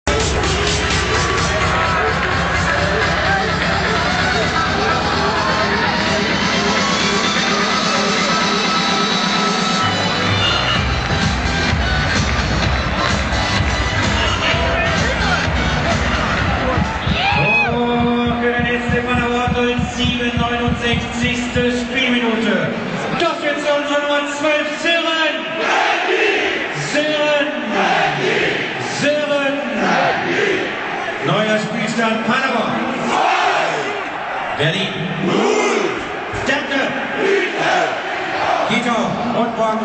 SCP-Union Berlin 2. Treffer
Das 2:0 durch Sören Brandy (: